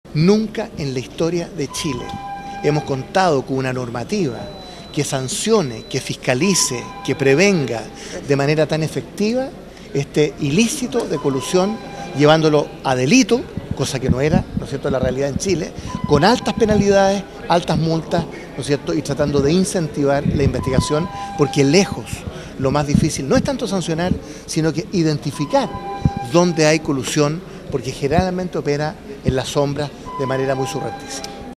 Senador Ignacio Walker destaca falta de normativa que sancione casos de colusión
El parlamentario dijo que este delito debiera sancionarse con altas penalidades y multas y agregó que hay que incentivar la investigación de estos casos porque lo complejo no es sancionar, sinoidentificar dónde hay colusión.